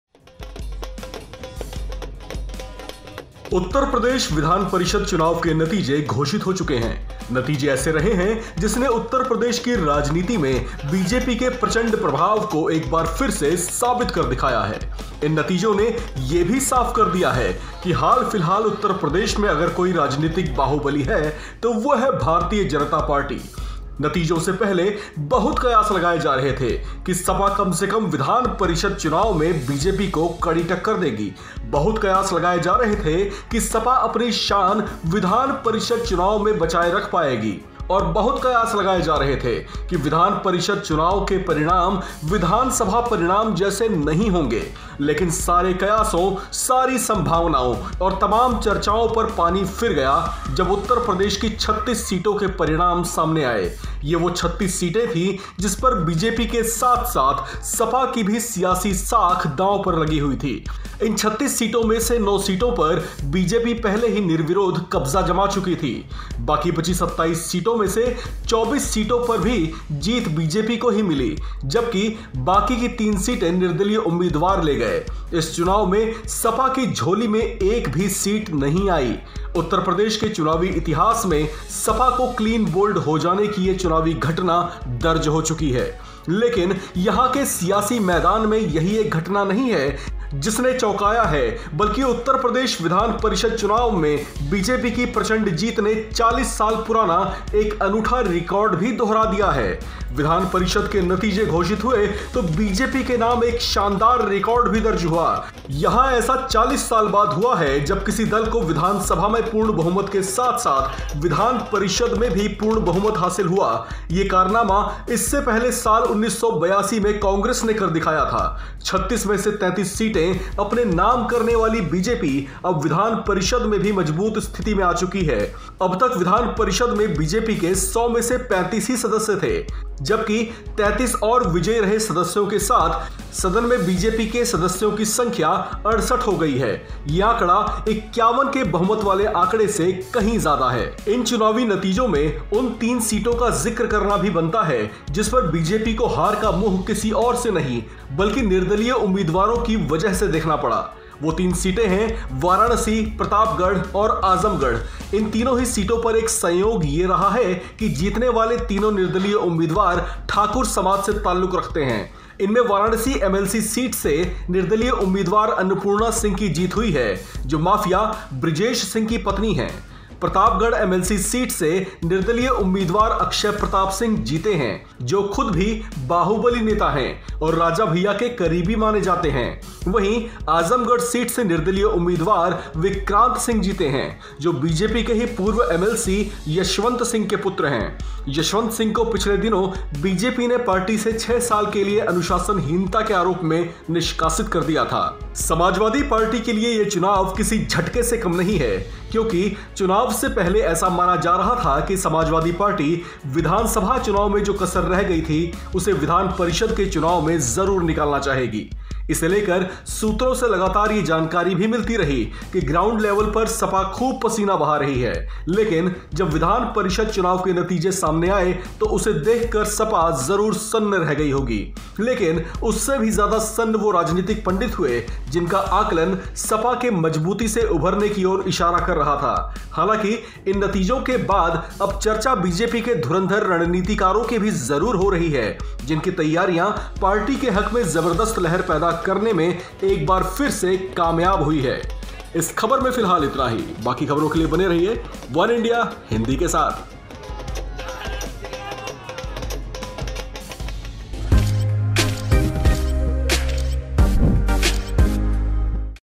UP MLC Election Results 2022: BJP ने 40 साल पुराने किस रिकॉर्ड की बराबरी की? | वनइंडिया हिंदी
The results of Uttar Pradesh Legislative Council elections have been declared. The results have been such, which has once again proved the tremendous influence of BJP in the politics of Uttar Pradesh. These results have also made it clear that if there is any political power in Uttar Pradesh recently, it is the Bharatiya Janata Party. BJP's thumping victory in the Uttar Pradesh Legislative Council elections has also repeated a unique 40-year-old record.

उत्तर प्रदेश विधान परिषद चुनाव के नतीजे घोषित हो चुके हैं। नतीजे ऐसे रहे हैं, जिससे उत्तर प्रदेश की राजनीति में बीजेपी के प्रचंड प्रभाव को एक बार फिर से साबित कर दिखाया है। इन नतीजों ने ये भी साफ कर दिया है, कि हाल-फिलहाल उत्तर प्रदेश में अगर कोई राजनीतिक बाहुबली है तो वो है भारतीय जनता पार्टी। उत्तर प्रदेश विधान परिषद चुनाव में बीजेपी की प्रचंड जीत ने 40 साल पुराना एक अनूठा रिकॉर्ड भी दोहरा दिया है।